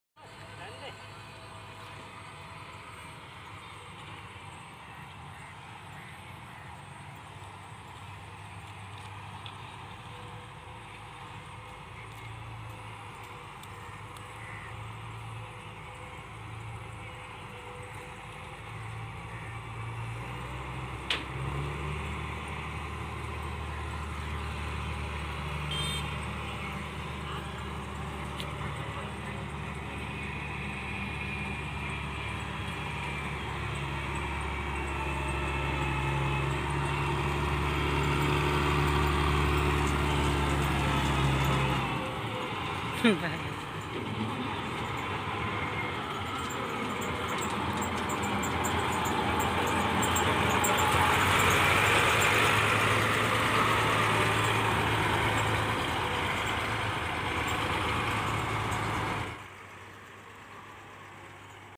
New combine harvester stuck in mud up down road | village2 tractor
Some interesting video from village2 tractor

Swaraj 735 FEe Stuck in Mud With Sand Loaded Trolley Rescued Out By Swaraj 735 Fe | tractor video